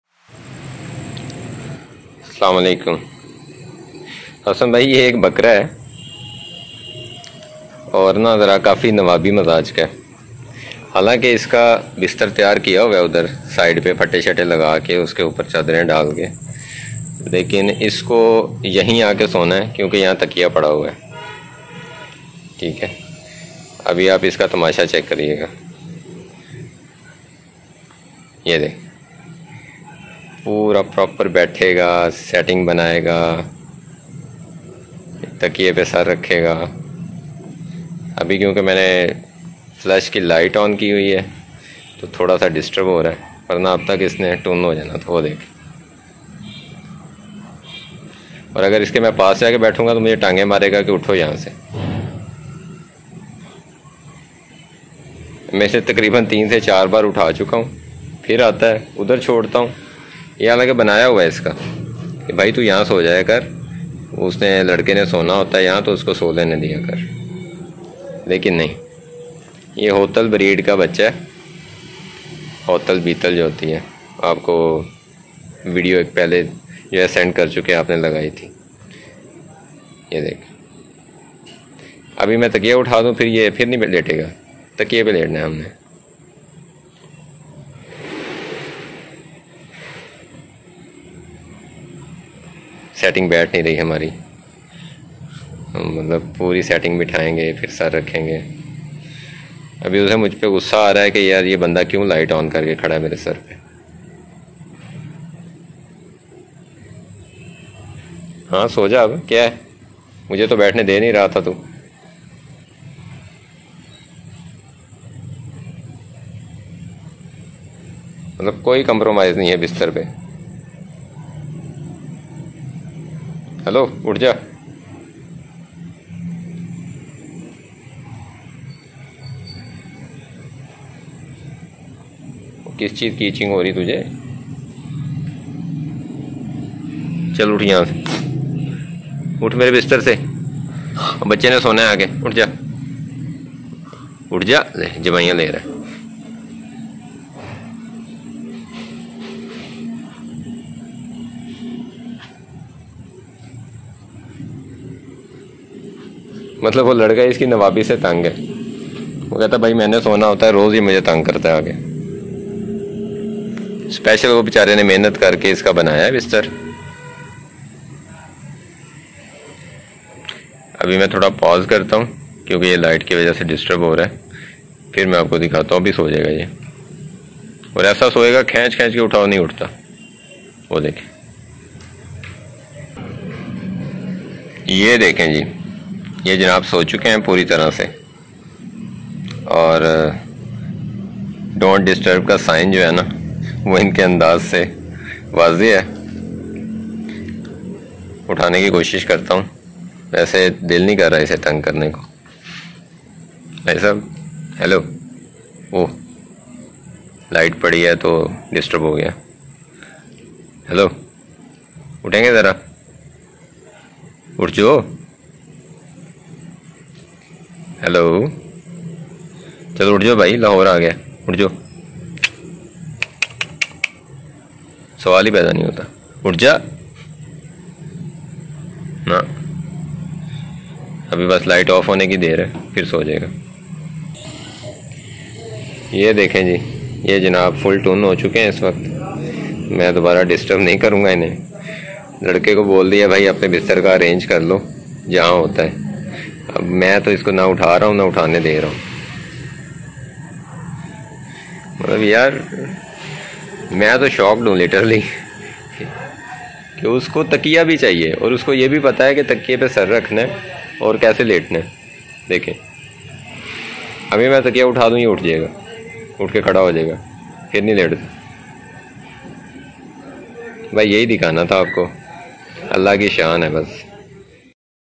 Goat Sleeping On Bed || بستر پہ سونے والا بکرا
آج کی ویڈیو میں ھم بستر پہ سونے والا بکرا دیکھے گے۔